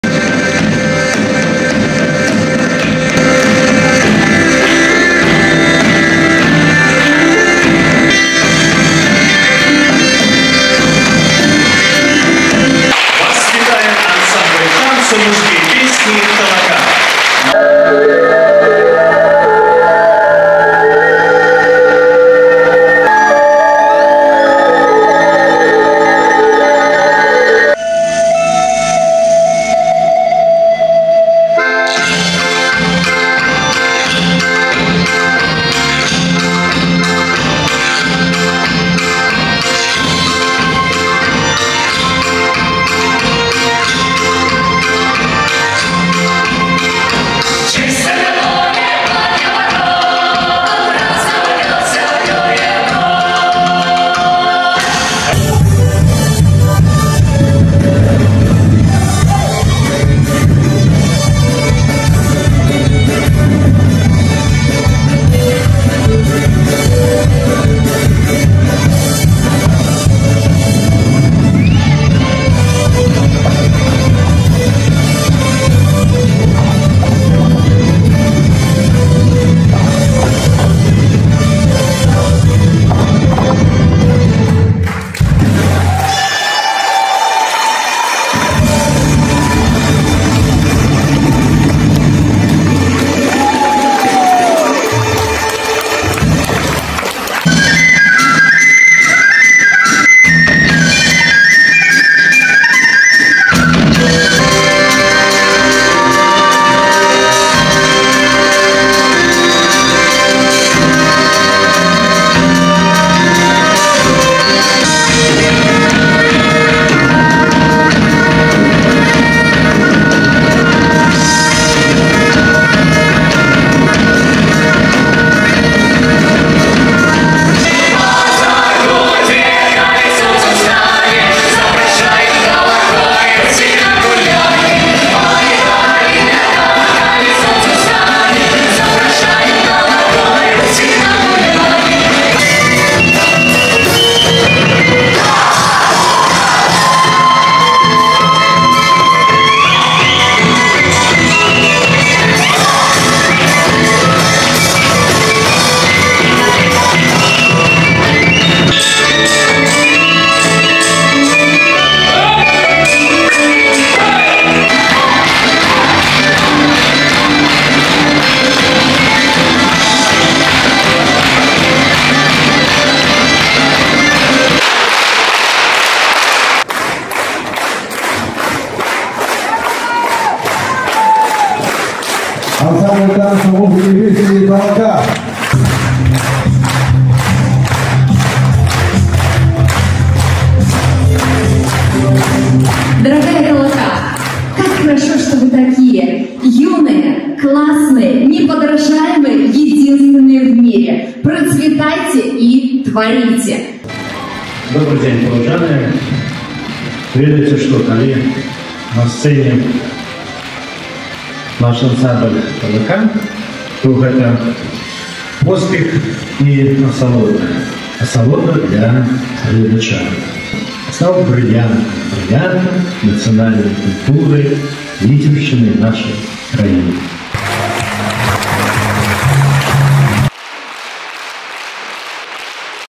ТАЛАКА - 30 лет. Юбилейный концерт в Витебске 2024
29 мая 2024 года, Витебская областная филармония.
Праздничный концерт, посвящённый 30-летию ансамбля танца, музыки и песни ТАЛАКА -"УСЛЕД ЗА СОНЦАМ".